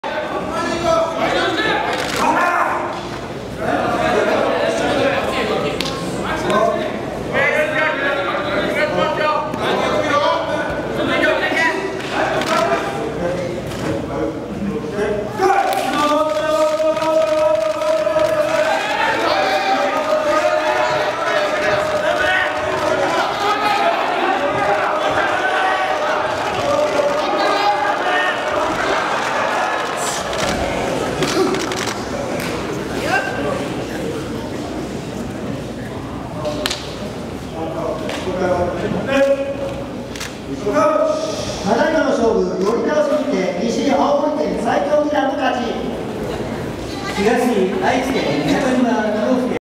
2013/09/30 相撲 国体 少年 個人 決勝T3 41 鈴木拓巳-斉藤学
鈴木拓巳-斉藤学